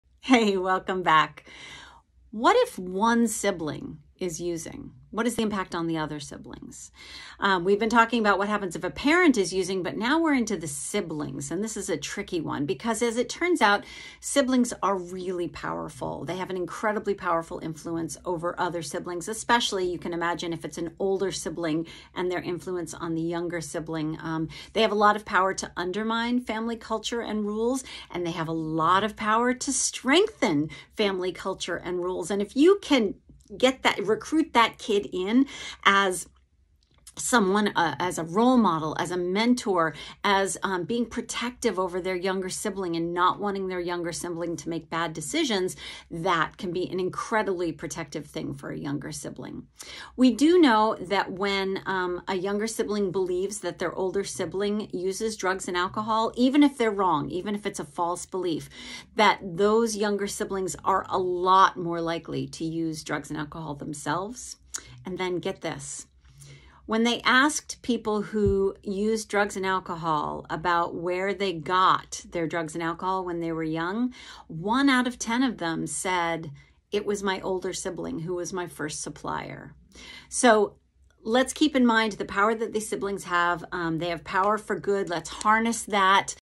Episode 81: Welcome back to The Addiction Inoculation in My Office!
Episode of 81: In which we continue to make our way through my book, The Addiction Inoculation: Raising Healthy Kids in a Culture of Dependence, and learn how to prevent substance use in kids